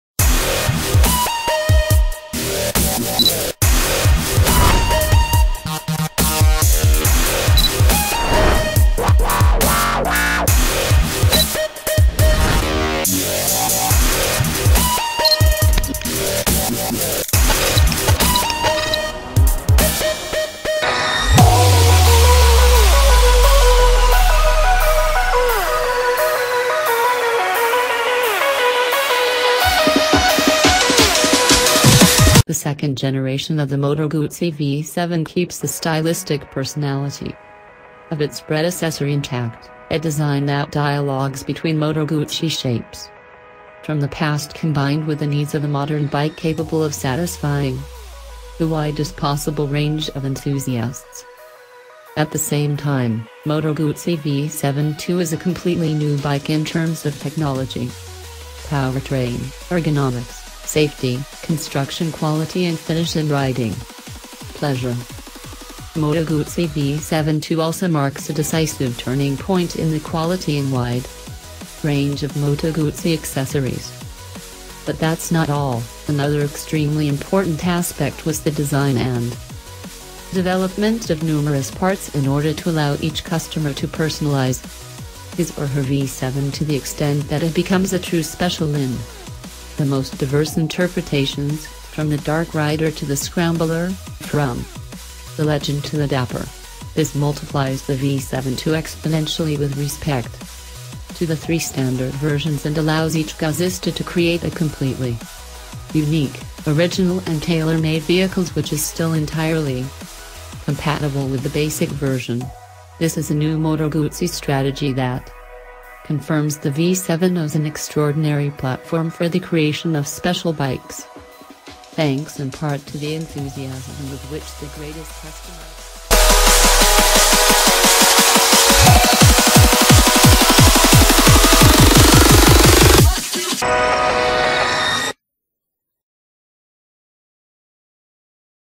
MUST WATCH !!! 2017 Moto Guzzi V7 II Special PRICE & SPEC
MUST WATCH !!! 2017 Moto Guzzi V7 II Special PRICE & SPEC - The second generation of the Moto Guzzi V7 keeps the stylistic personality
 of its predecessor intact: a design that dialogues between Moto Guzzi shapes
 from the past combined with the needs of a modern bike capable of satisfying
 the widest possible range of enthusiasts.

$7990